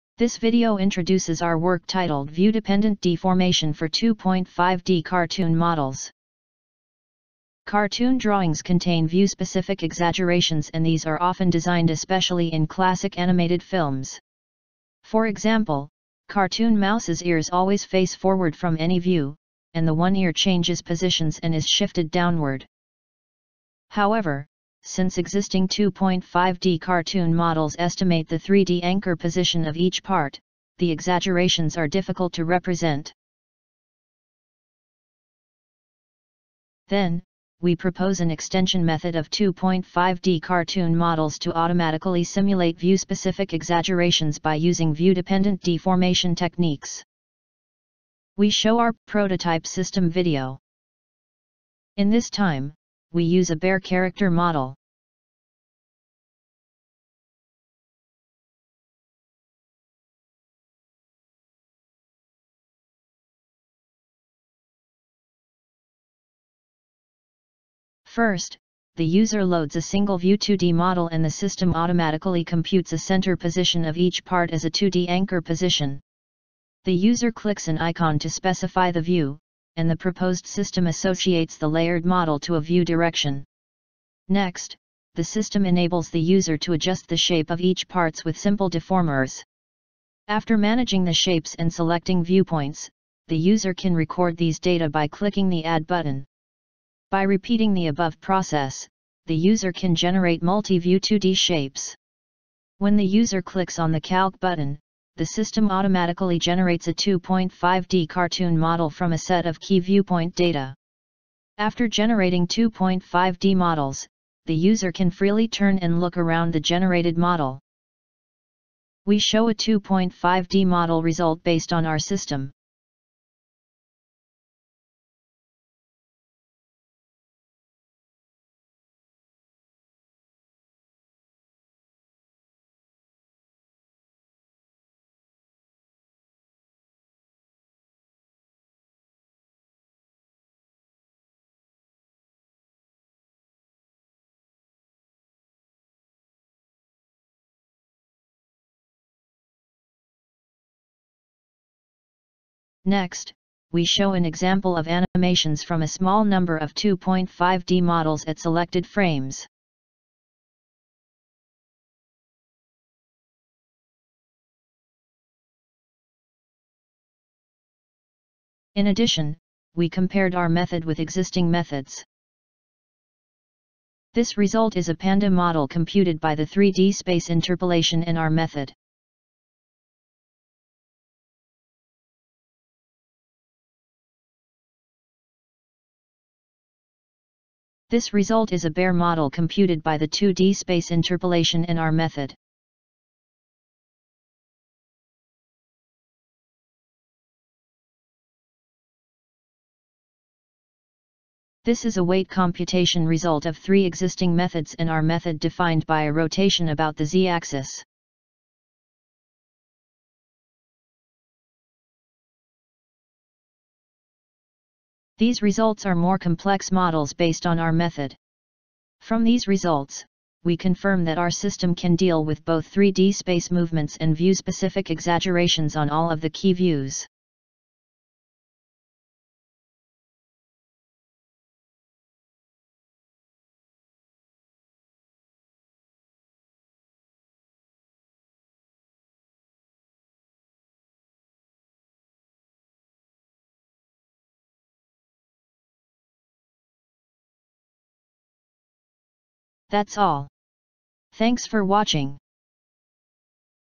View-Dependent Deformation for 2.5D Cartoon Models
2.5D cartoon models are popular methods used for simulating three-dimensional (3D) movements, such as out-of-plane rotation, from two-dimensional (2D) shapes in different views without 3D models. However, cartoon objects and characters have several exaggerations that do not correspond to any real 3D positions (e.g., Mickey Mouse’s ears), which implies that existing methods are unsuitable for designing such exaggerations. Hence, we incorporated view-dependent deformation (VDD) techniques, which have been proposed in the field of 3D character animation, into 2.5D cartoon models. The exaggerations in an arbitrary viewpoint are automatically obtained by blending the user-specified 2D shapes of key views. Several examples demonstrated the robustness of our method over previous methods. In addition, we conducted a user study and confirmed that the proposed method is effective for animating classic cartoon characters.